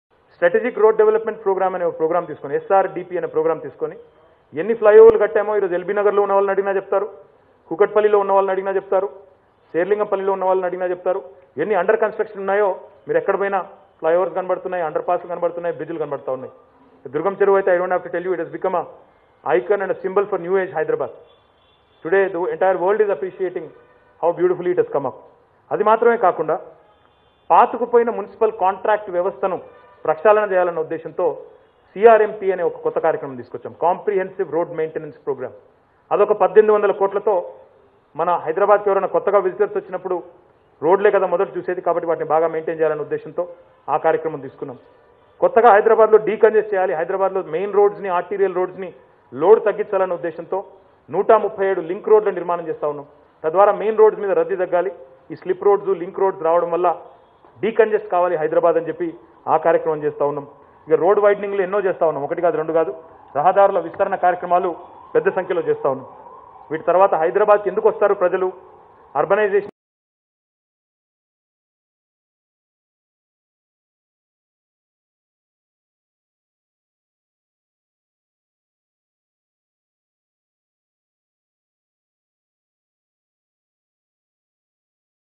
Many flyovers have been constructed by SRDP || Minister KTR Press meet || Hybiz
SRDP ద్వారా ఎన్నో ఫ్లైఓవర్ లు నిర్మించాం || Minister KTR Press meet || Hybiz